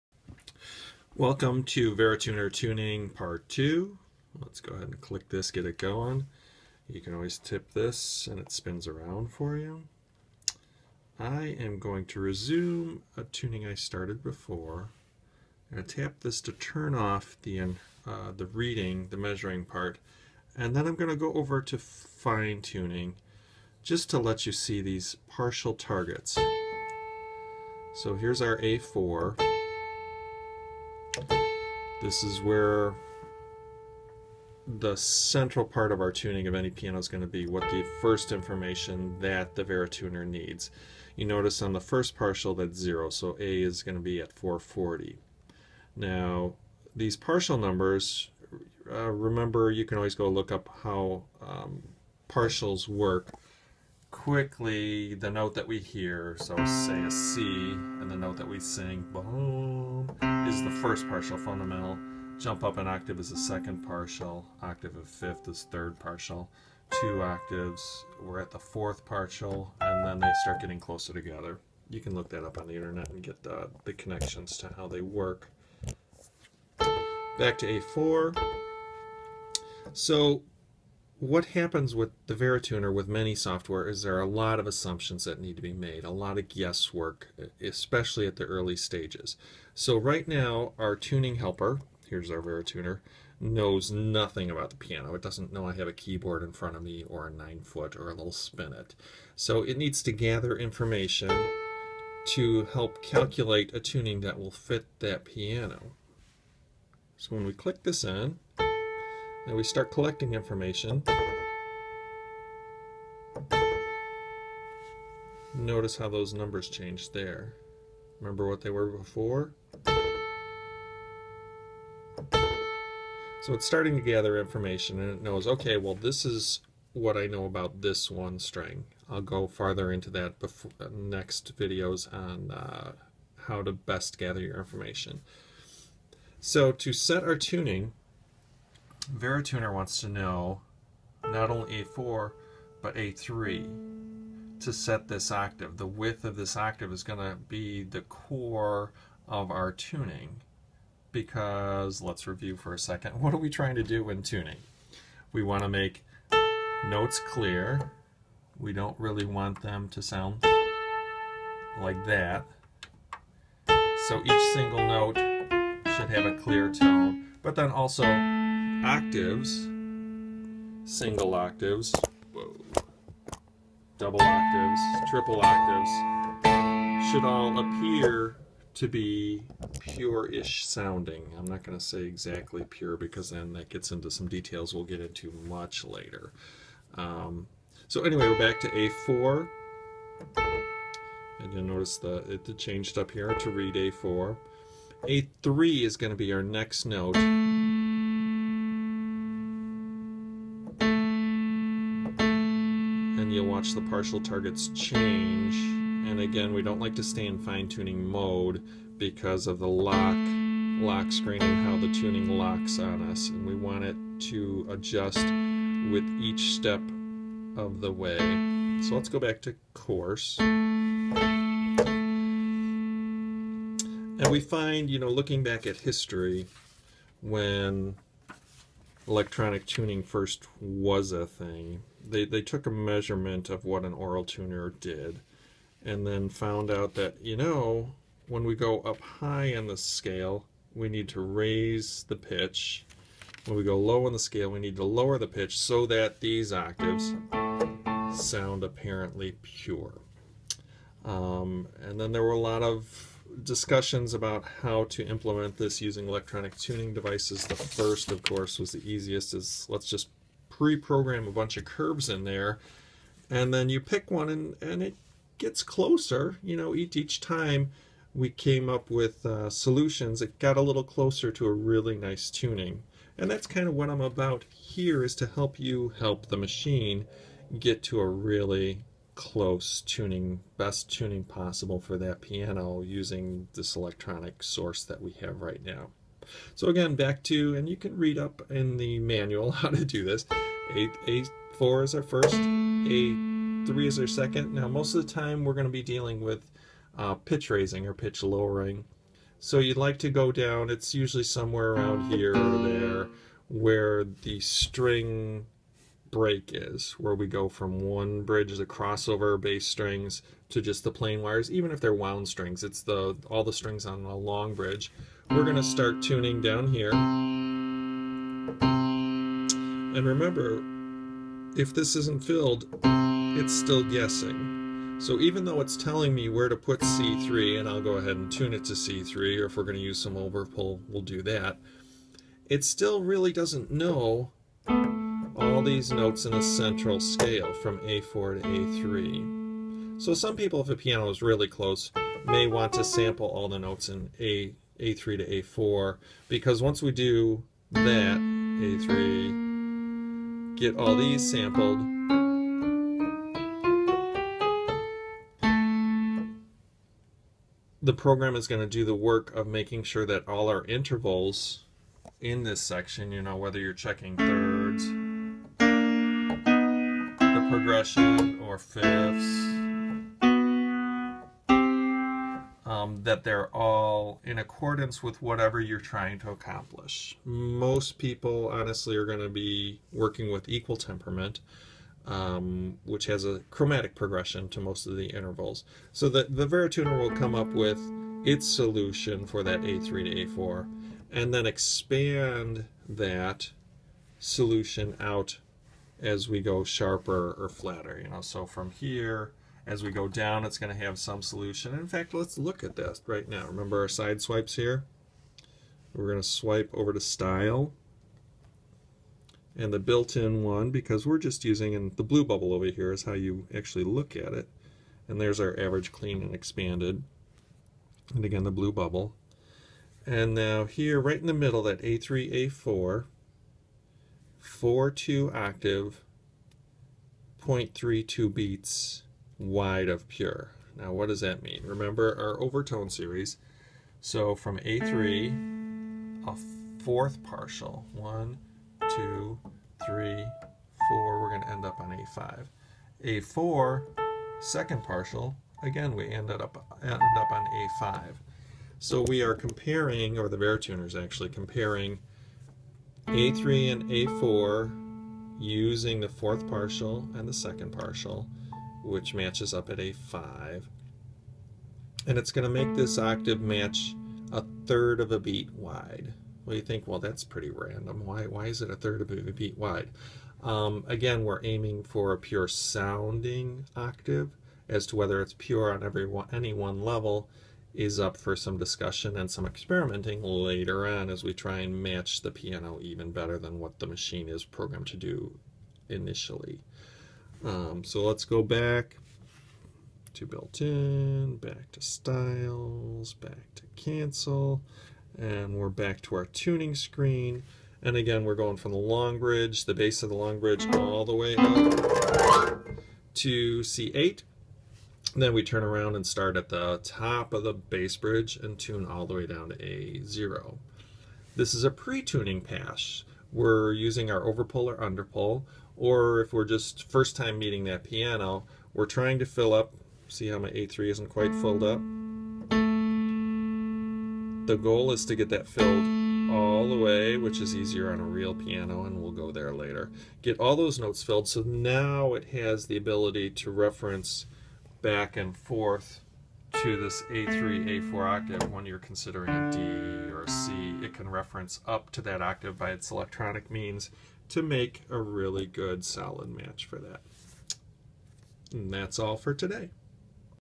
Verituner basics 2
Beginning steps to using software tool for piano tuning.